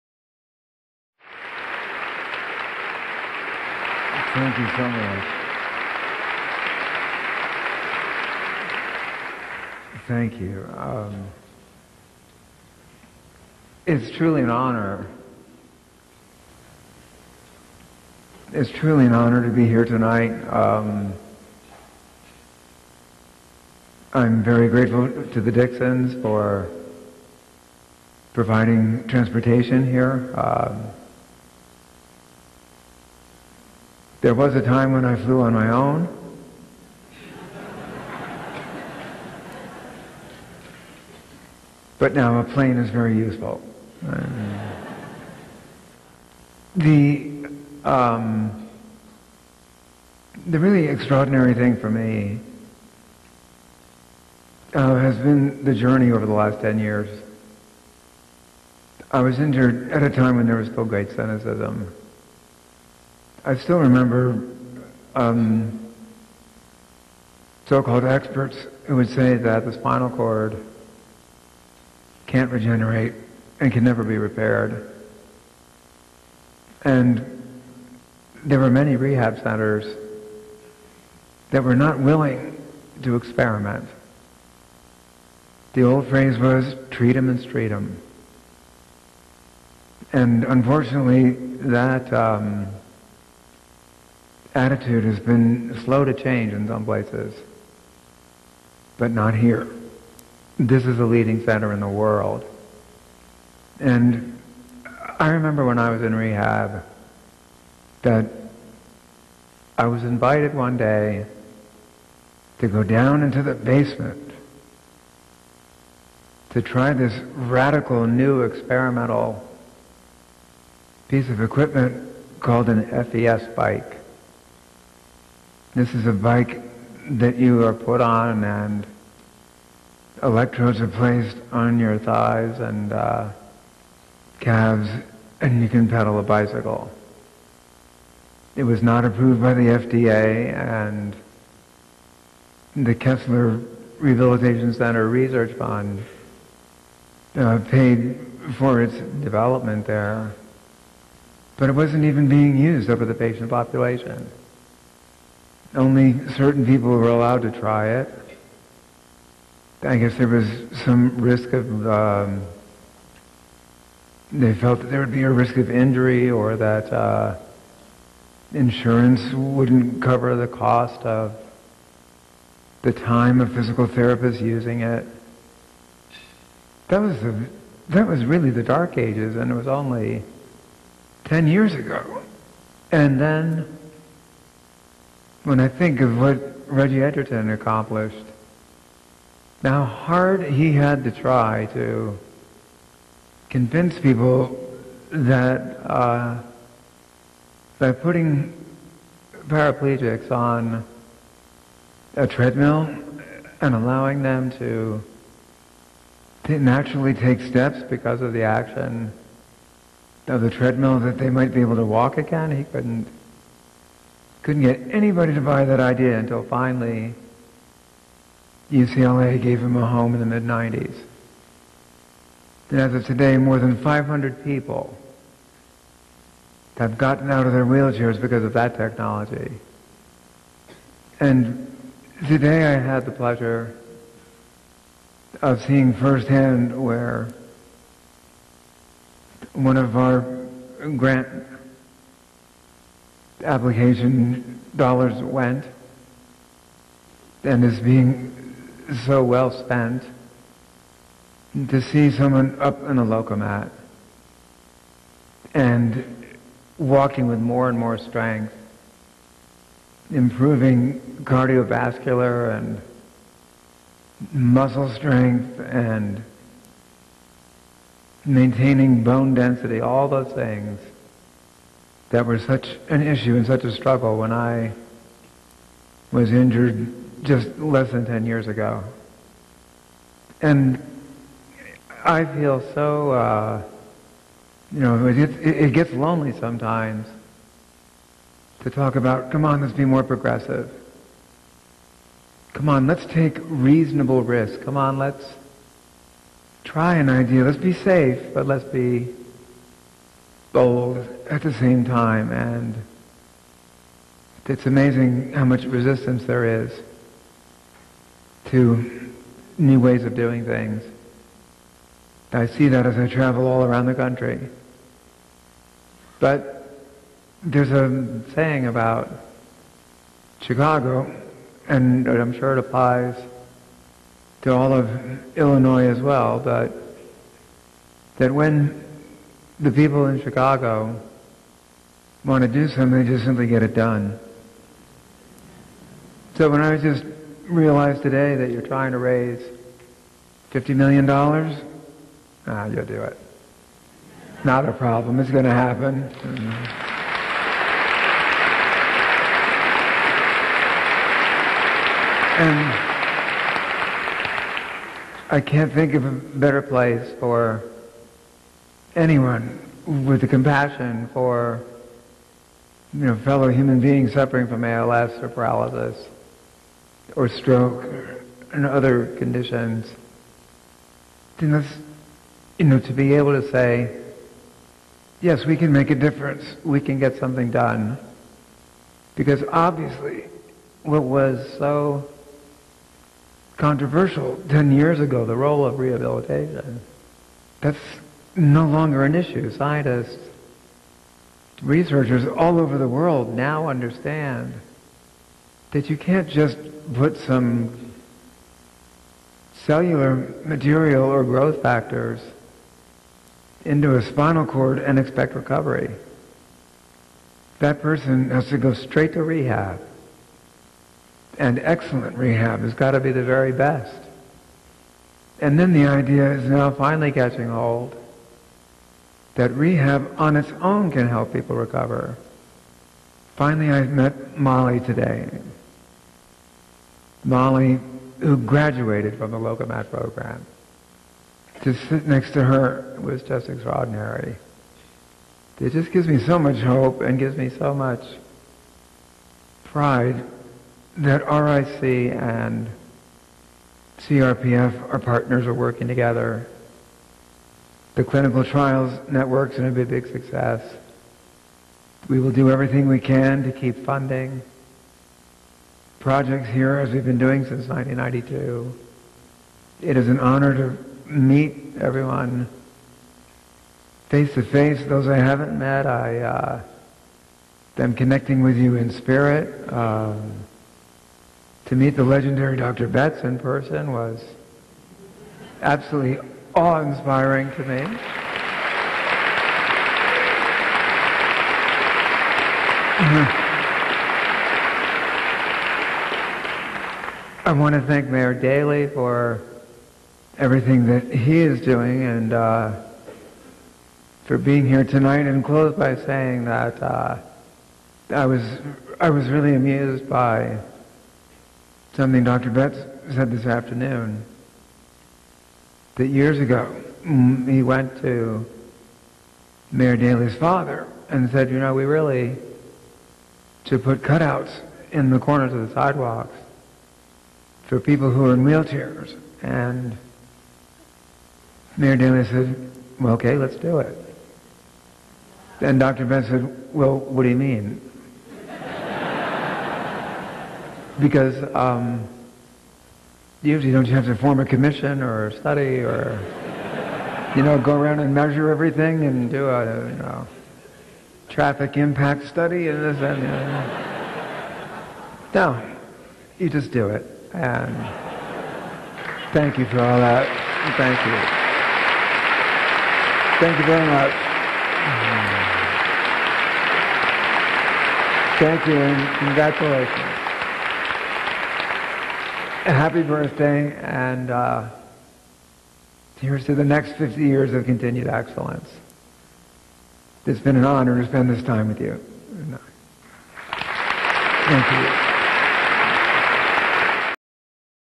CHIRSTOPHER REEVE LAST PUBLIC SPEECH AT REHABILITATION INSTITUTE OF CHICAGO 10-04-2004
HE USED TO HAVE SO MUCH COURAGE THAN NO ONE I EVER KNOW.  I WILL LOVE SUPERMAN FOR EVER.